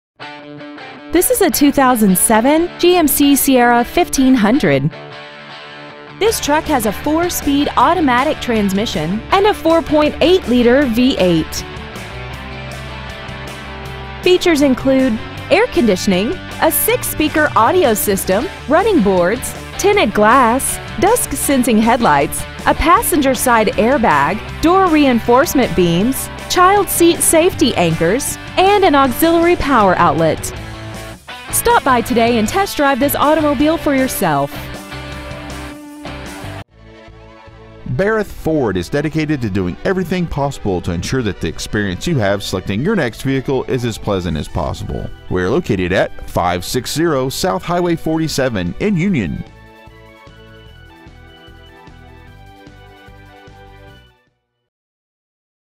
Used 2007 GMC SIERRA 1500 Union MO
We have been honored to serve the Union MO area , we promise that your experience at our dealership will exceed your expectations !

 Year : 2007
 Make : GMC
 Model : SIERRA 1500
 Engine : 4.8L V8 16V MPFI OHV
 Trans . : 4-SPEED AUTOMATIC
 Exterior : SUMMIT WHITE
 Miles : 99,894
 Interior : EBONY
 Stock : 13088B